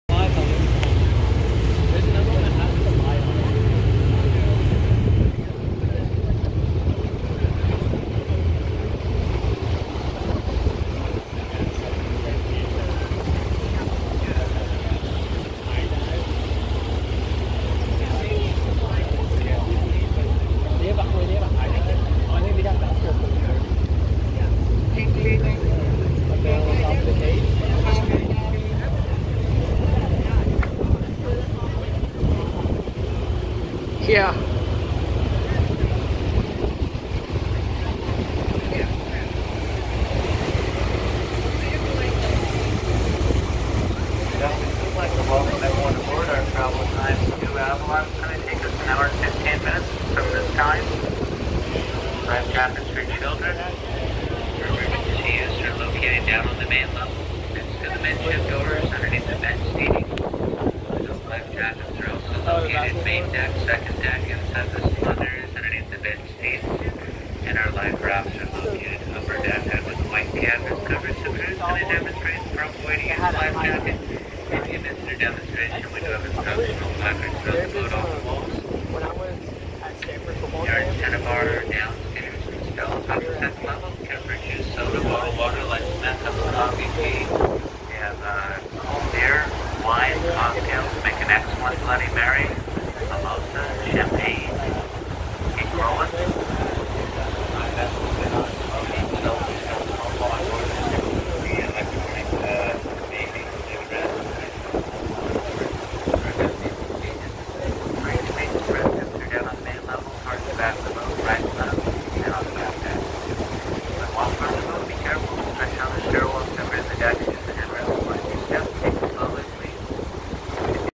Catalina Express Ferry Ride Departure from San Pedro to Catalina Island - August 13, 2022
Catalina Express ferry ride departure from San Pedro to Avalon, Catalina Island. Here are the views of the Vincent Thomas Bridge and the cargo containers from the Port of Los Angeles in San Pedro.